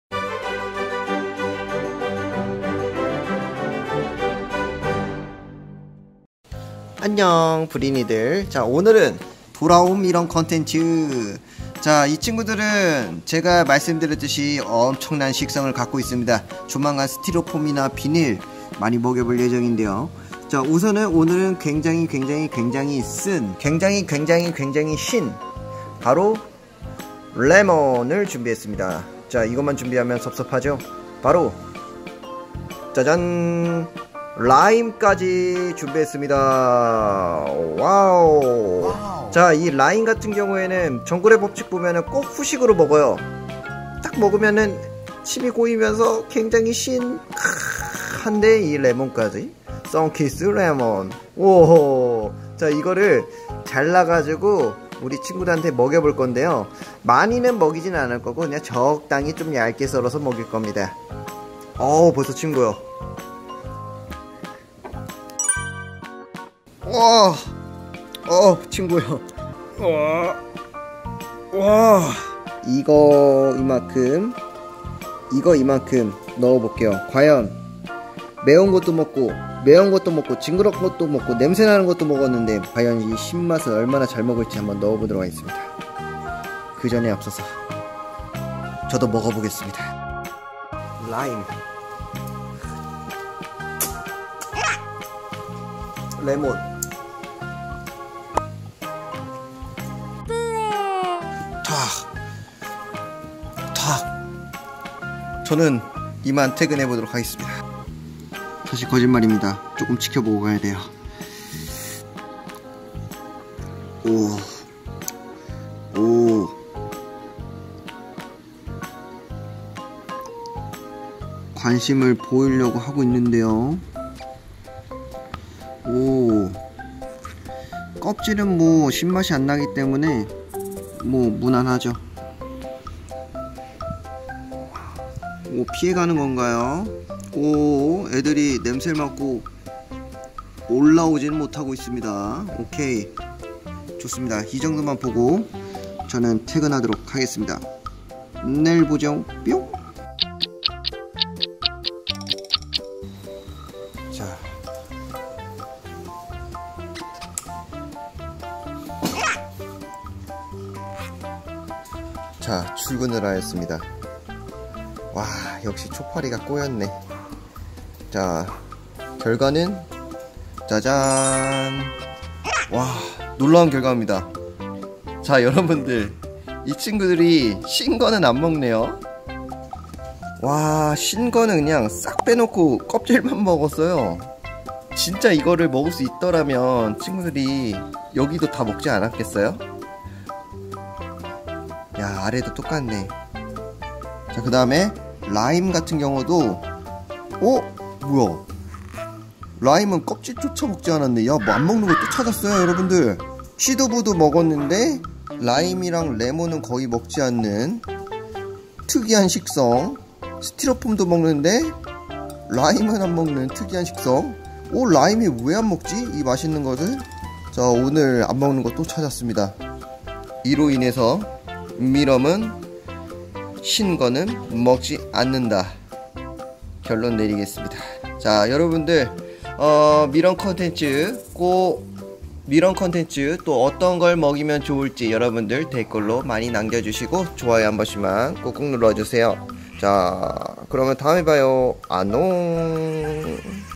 밀웜5천마리에게 "라임"을 줘봤는데....역대급반전ㅋㅋㅋ첨임 [정브르]
와 정말이지...뭘 먹고 뭘 못 먹는지 감이 안잡히는데...
휴지랑 A4용지도 먹을까... 비닐도 먹을까..
환경을 살릴수 있는 비밀의 열쇠가 되는걸까??